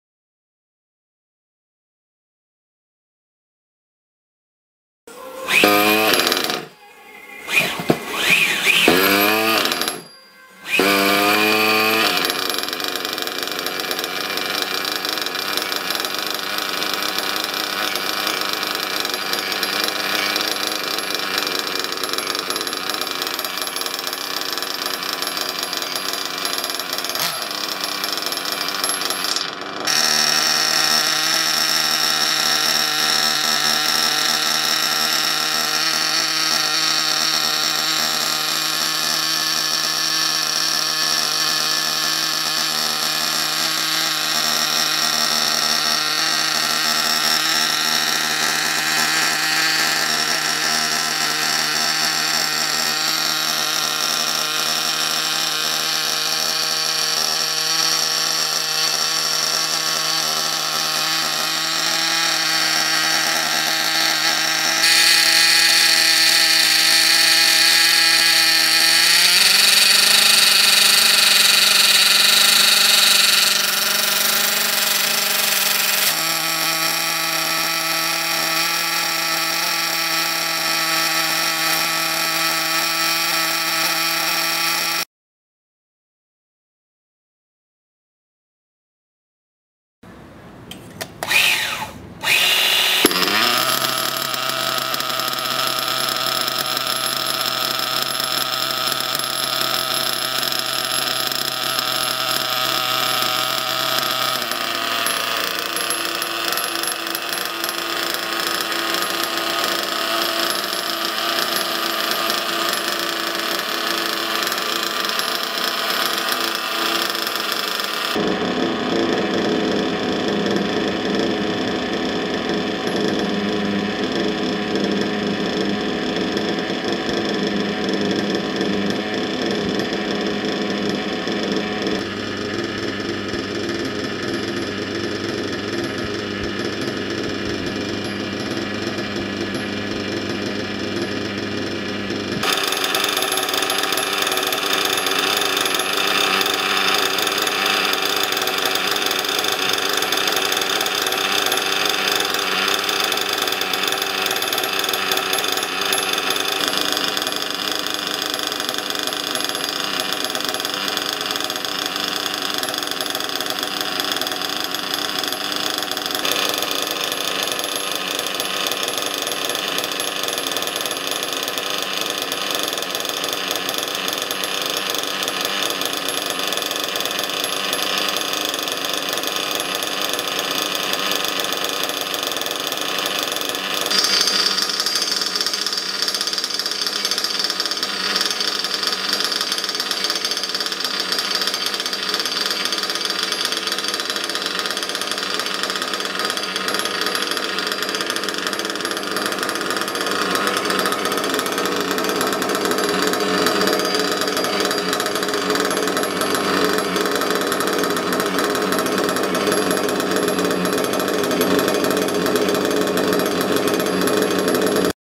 engine Reds XR21 break-in...
My experience with this new engine...